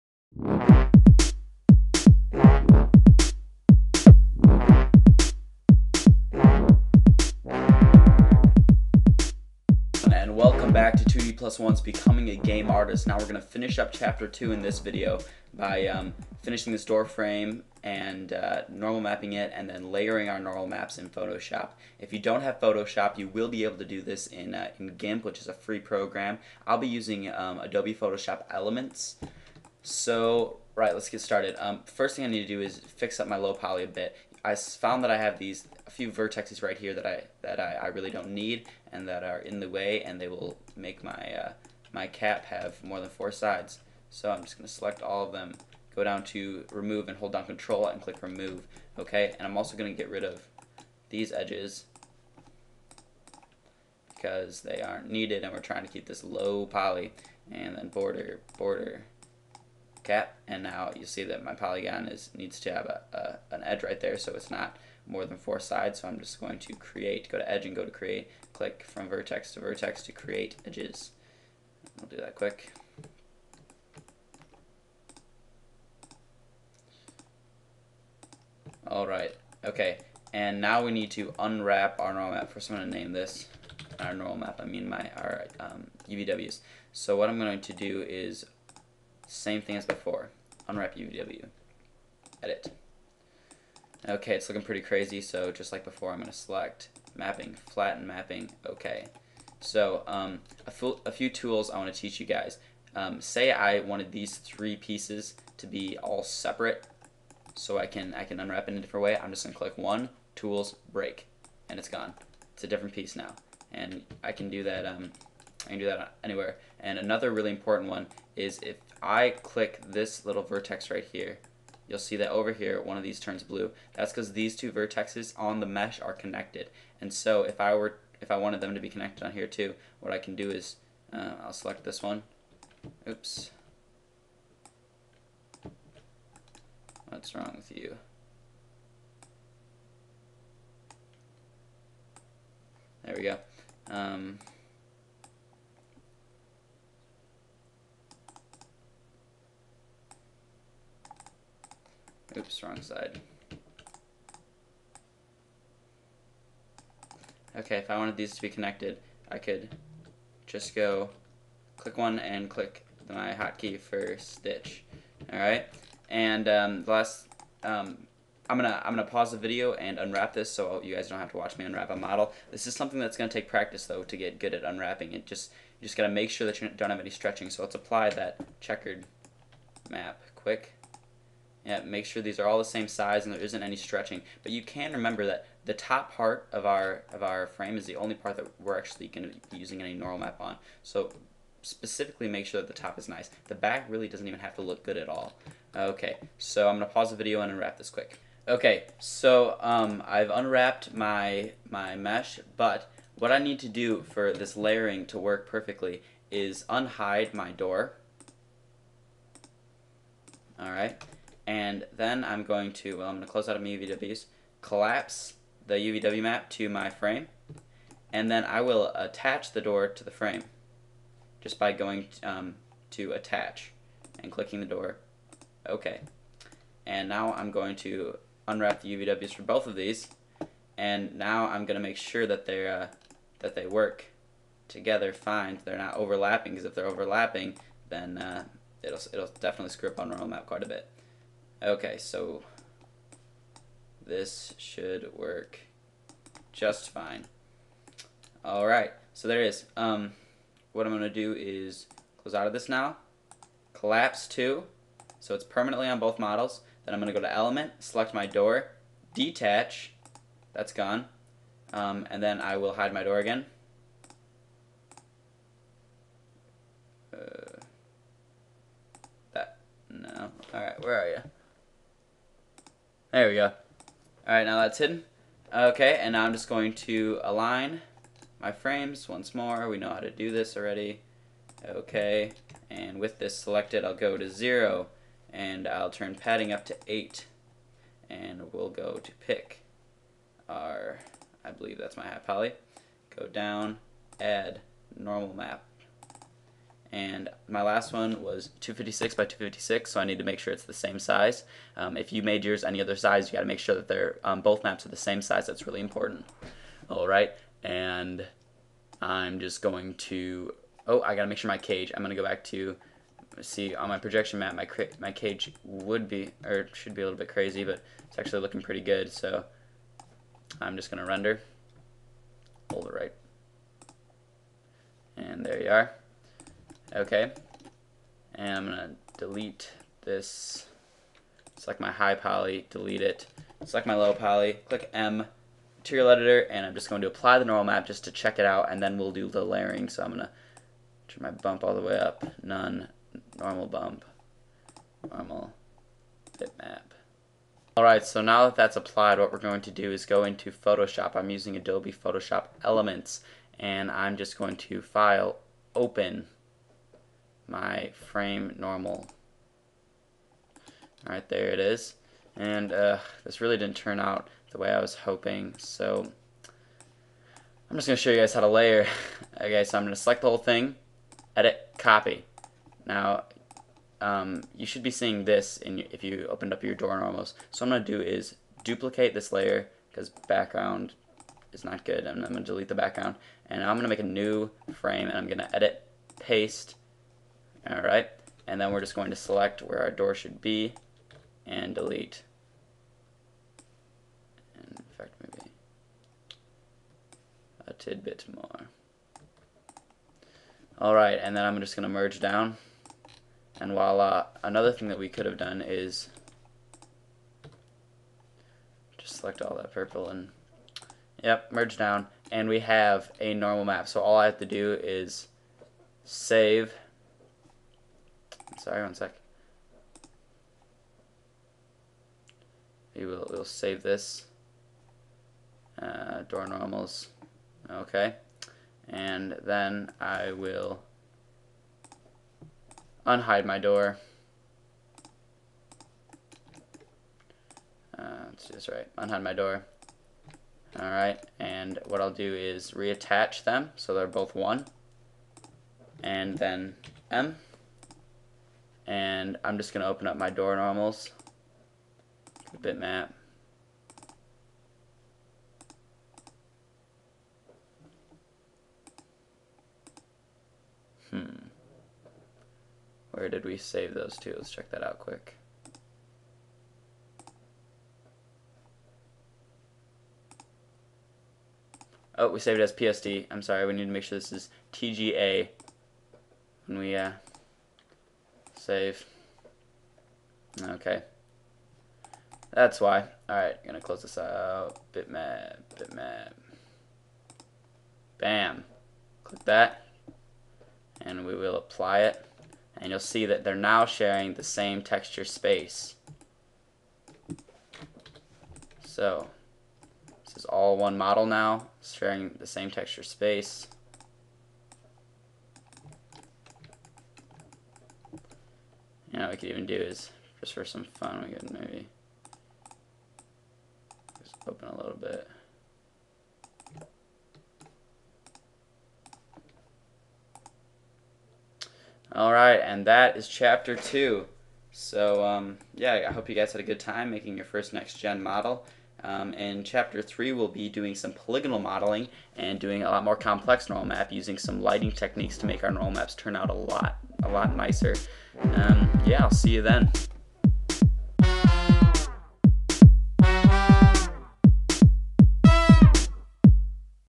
Becoming a game artist Chapter two part 4: Layering normals
In this video we layer our normal maps so our two objects baked at different times, share on normal map.